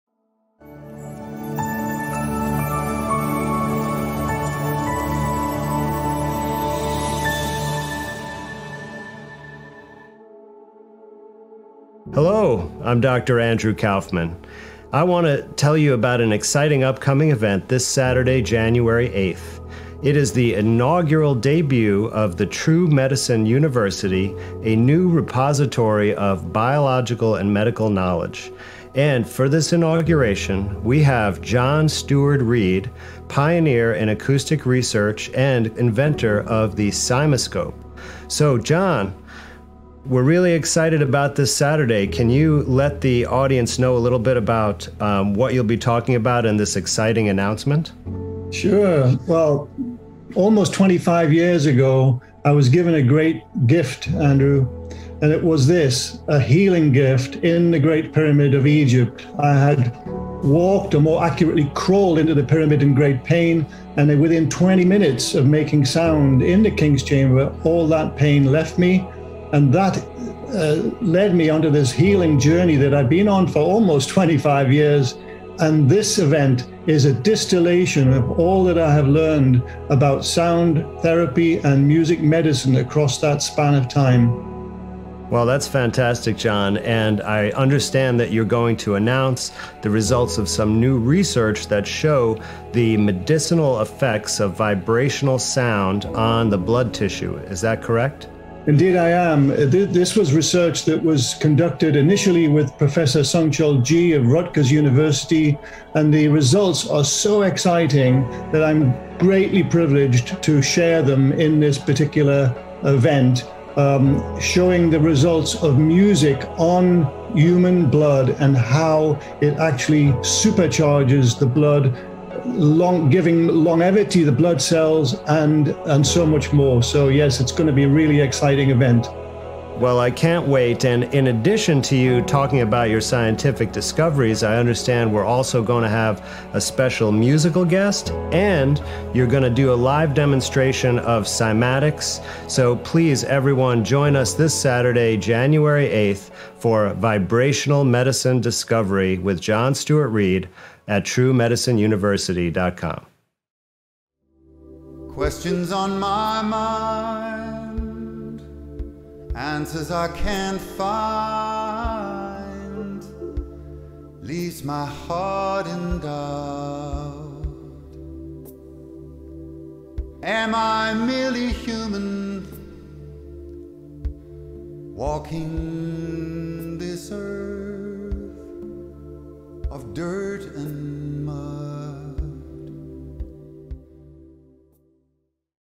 John Stuart Reid and Andrew Kaufman M.D. Vibrational Medicine Discovery by True Medicine University
ANDREW KAUFMAN'S TRUE MEDICINE UNIVERSITY
PRESENTS: THE WONDERS OF SOUND AND CYMATICS: VIBRATIONAL MEDICINE DISCOVERY

SATURDAY JANUARY 8TH 12PM EST

Acoustics pioneer, John Stuart Reid, presents the Wonders of Vibrational Medicine and Cymatics, including the first-time announcement of his extraordinary blood-based breakthrough discovery that occurred during his collaboration with Professor Sungchul Ji of Rutgers University.
Hosted by Dr. Andrew Kaufman with musical interludes from internationally renowned musicians, Anders Holte and Cacina Meadu.

Join us to learn pioneering insights into Vibrational Medicine with the power to enhance your life and bring about healing, from a world-leading authority on Cymatics and Sound Therapy.

Don’t miss out on this once in a lifetime opportunity to learn some of the amazing healing powers that sound, frequency and vibration really have!

Watch Free: The Sound of Music Medicine of the Future
Watch a premiere replay of John Stuart Reid’s presentation recorded live at the True Healing Conference.

In his illuminating 60 minute presentation John Stuart Reid will distill for us some of the important biological mechanisms discovered during his 20 years of research and will share with us his intuitive sense that the Sound of Music will become The Medicine of the Future.